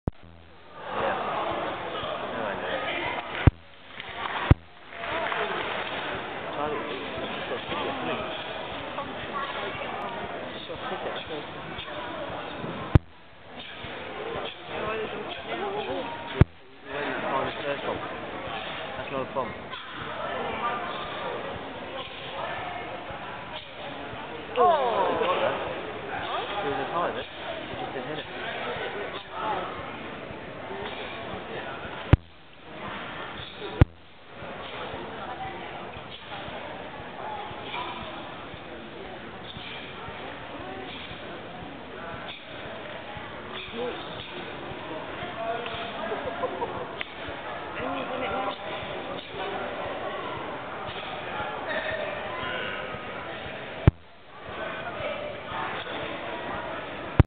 Mr Slater JNR Ge Baek
Mr Slater JNR of GTI Taekwondo performing pattern Ge-Baek at the GTI UK Open 2009